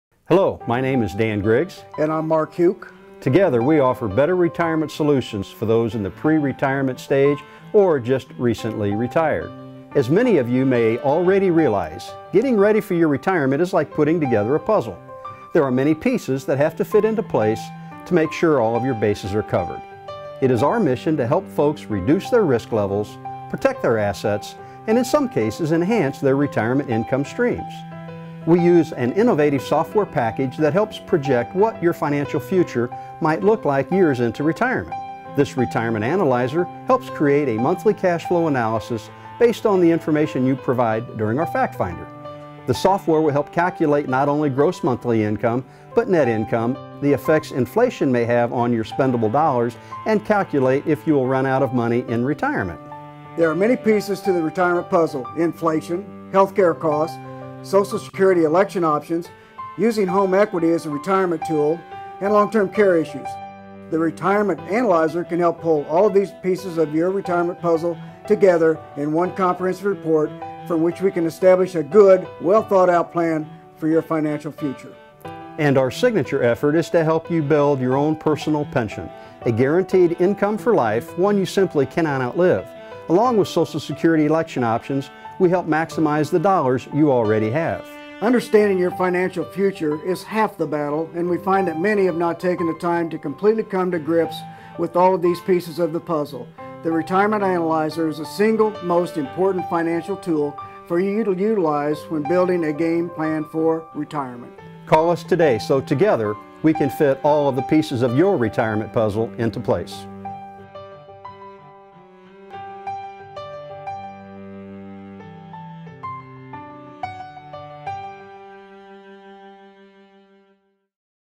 Better Retirement Solutions | FINDitKC
Better Retirement Solutions offers professional advice to clients with the understanding that everyone's needs are different.  Their Philosophy is "Client First".  Services include all of forms of Health and Life Insurance, annuities and disability income.  They focus on pre-retirement, planning, Social Security maximization, Medicare, Long Term Care and Small Business Benefits.